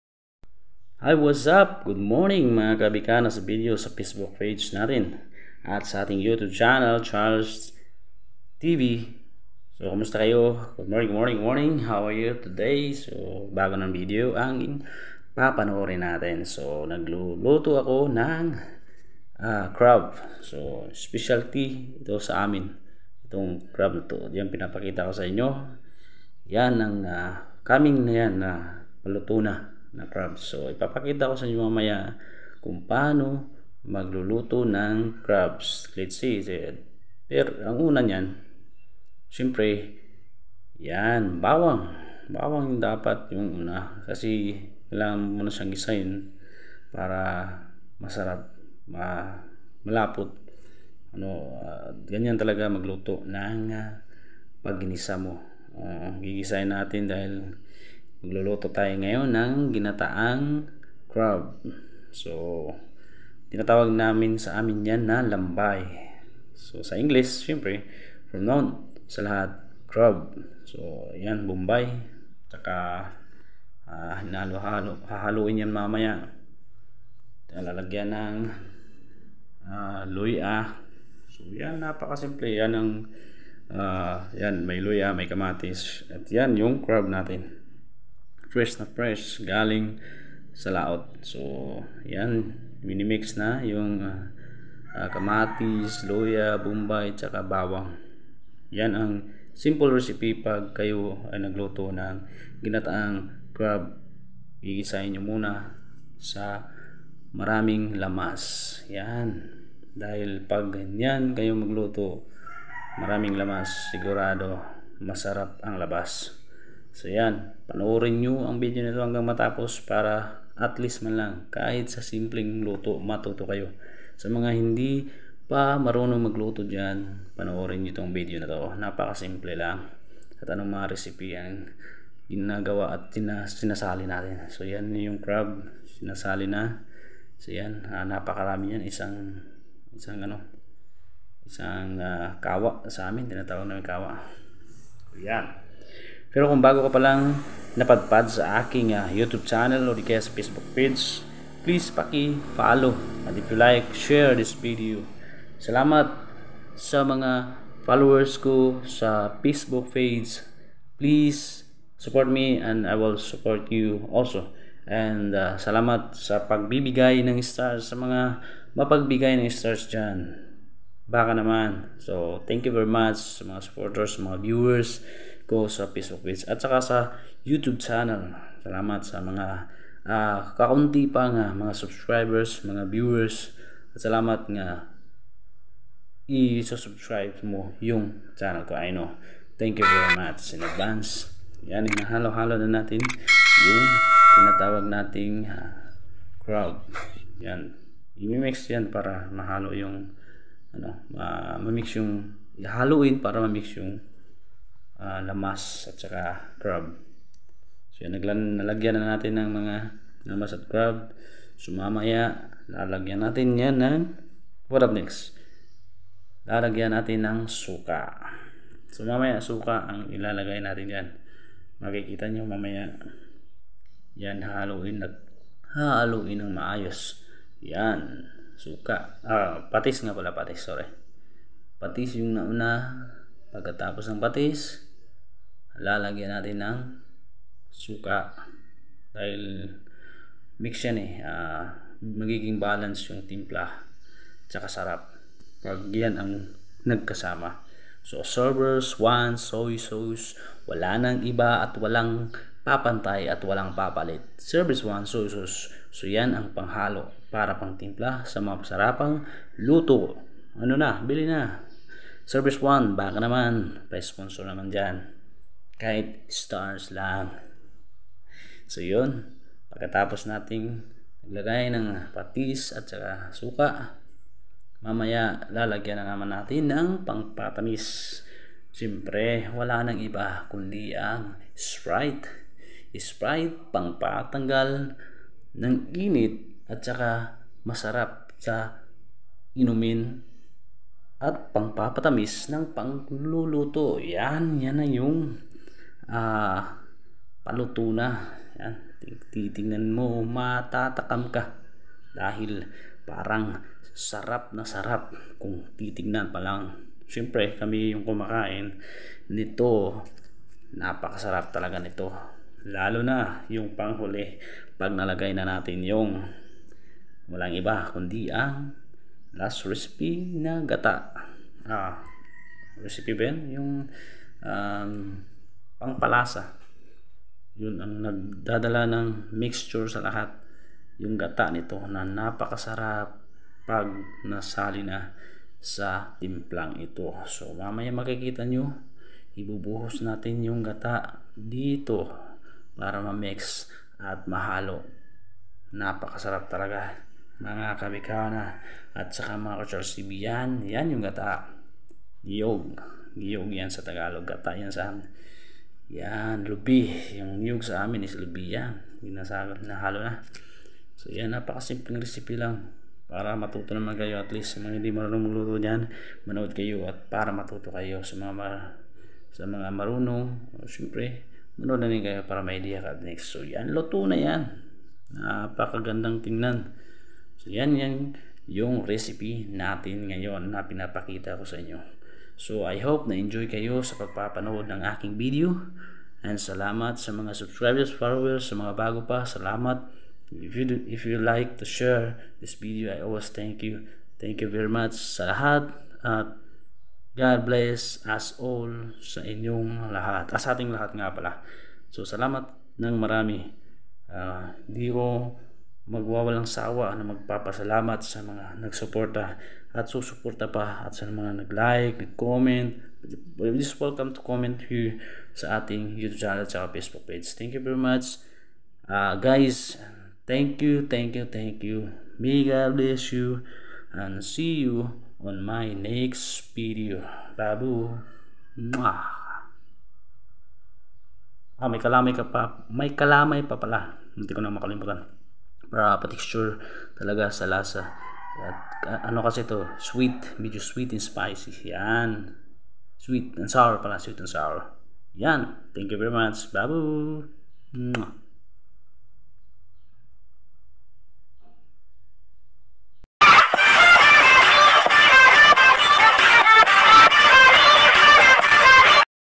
Pano ba mag loto nang ginataang lambay. crabisland.
Napa ka simpling pag loto nang crab or lamabay kung pano nood na at taposin mo Ang video na to.